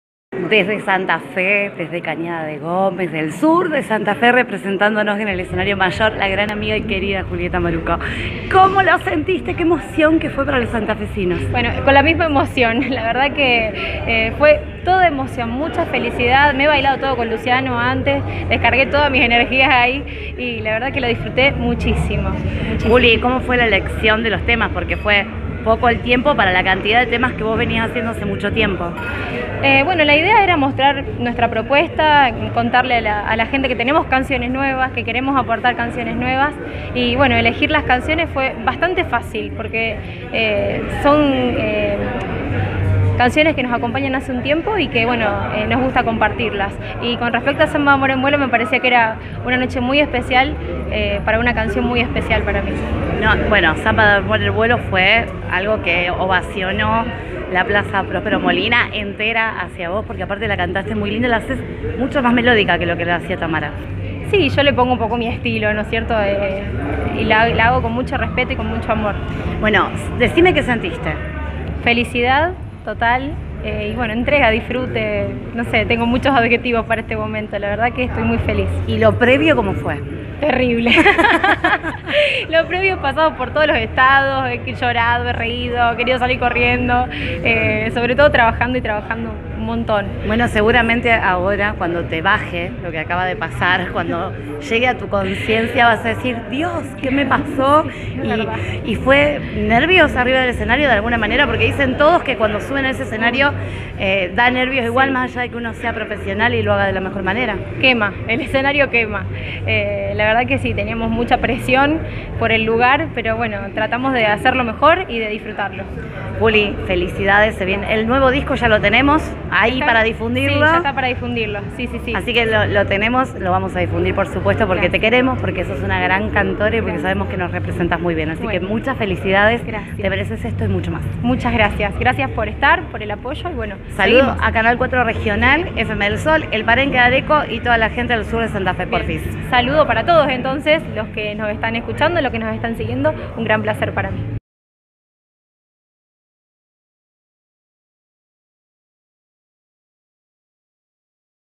Julieta Marucco brilló en el escenario mayor de Cosquín
La joven cañadense Julieta Marucco brindó un show impecable en la segunda noche del Festival Nacional de Folklore 2019.